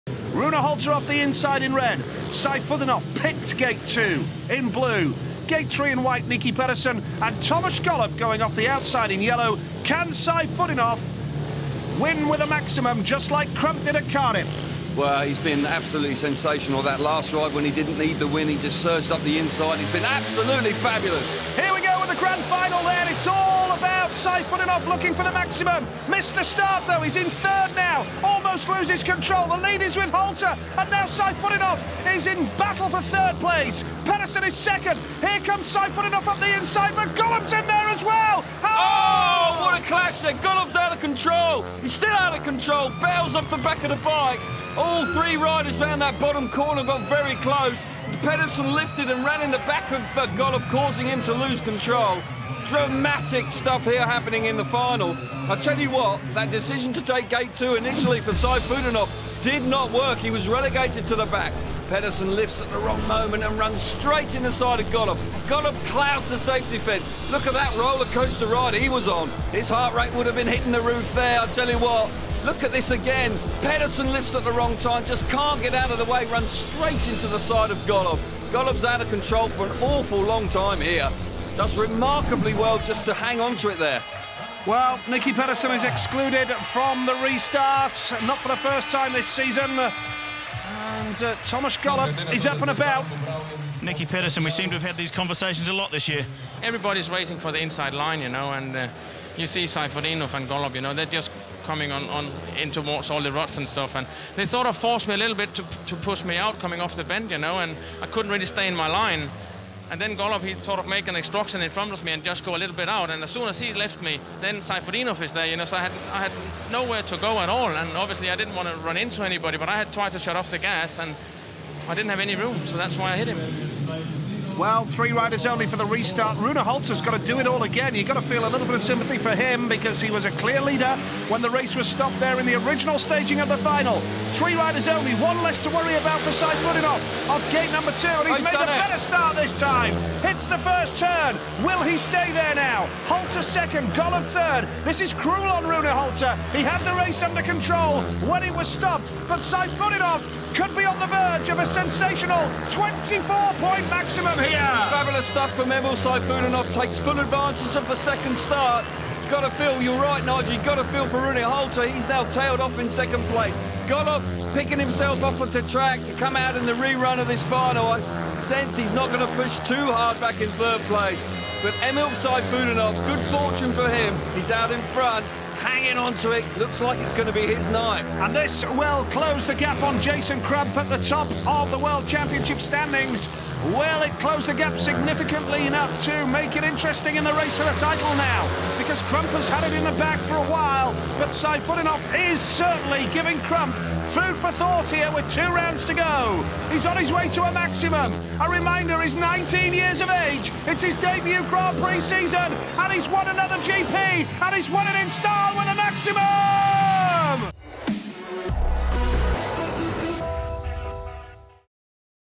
Emil's Krsko max in 2009 | SGP Throwback
We flashback 10 years to 2009 and a 24-point, seven-ride maximum for a 19-year-old Emil Sayfutdinov at the Slovenian SGP in Krsko.